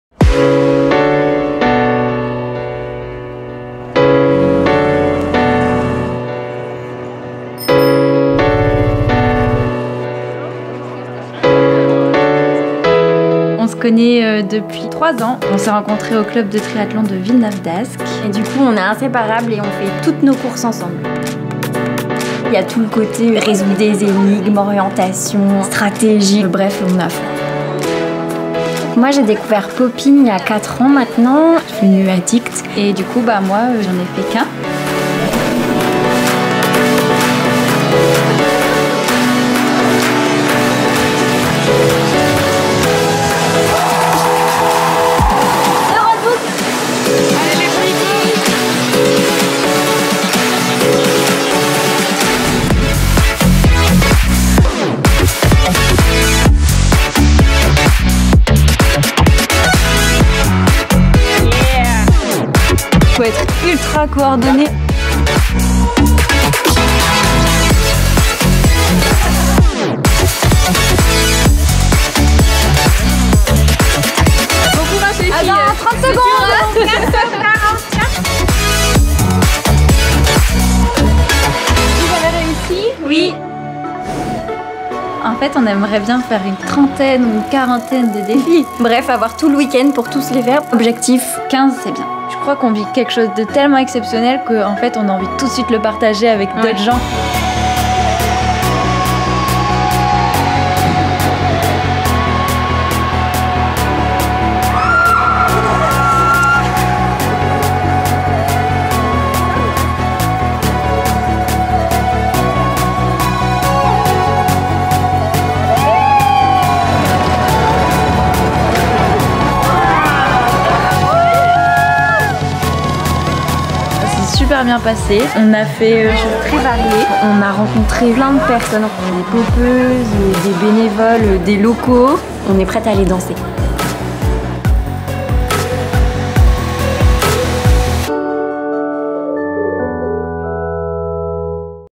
Pop In the City // Gand 2018
Faire de l'escrime dans la plus vieille salle du monde, repasser du pain avec un fer à repasser dans une abbaye, faire de l’handibasket, réaliser son propre Gin local et goûteux, se plonger dans le monde d’un autiste en étant perturbée par des stimuli sensoriels, dévaler une tyrolienne du 6ème étage d'un centre commercial, réaliser une fresque géante en graffiti...

Heaume sur la tête et armure sur les épaules, Pop In Gent ça donne ça !